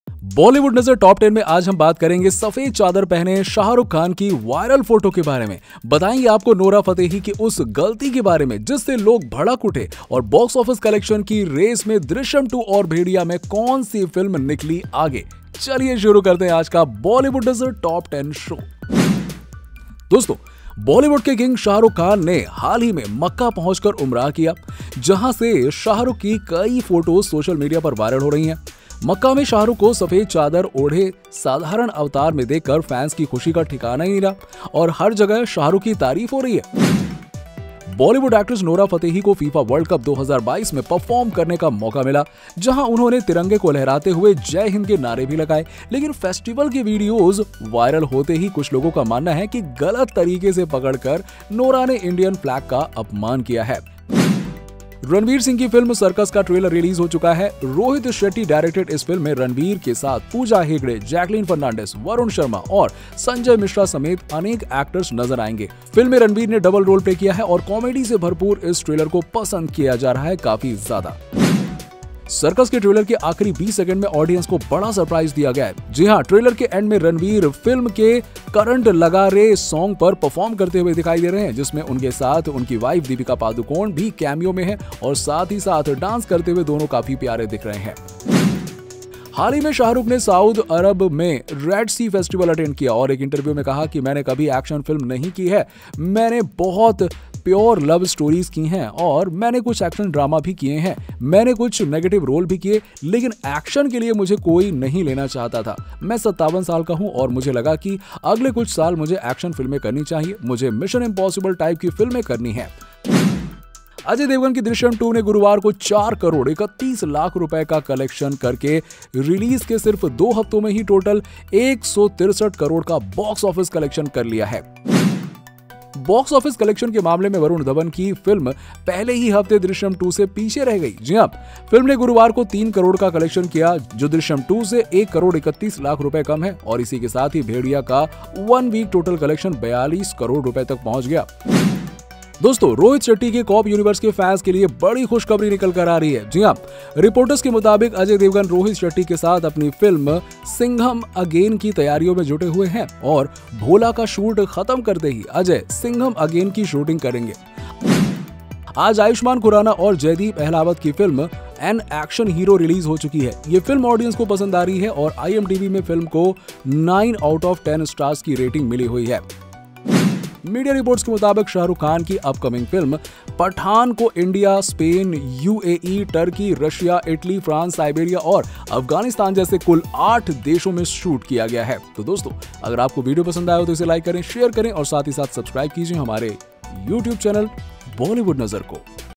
Nora Fatehi ने गलत तरीके से लहराया तिरंगा? | Shahrukh Khan ने किया Umrah | Bollywood Nazar Top 10
In this video of Bollywood Nazar Top 10, we will talk about Nora Fatehi Flag Controversy, Shahrukh Khan Umrah, Shahrukh Khan on Being action hero in Pathaan, Cirkus Teaser Deepika Padukone Cameo, Rohit Shetty and Ajay Devgn’s Singham Again and more.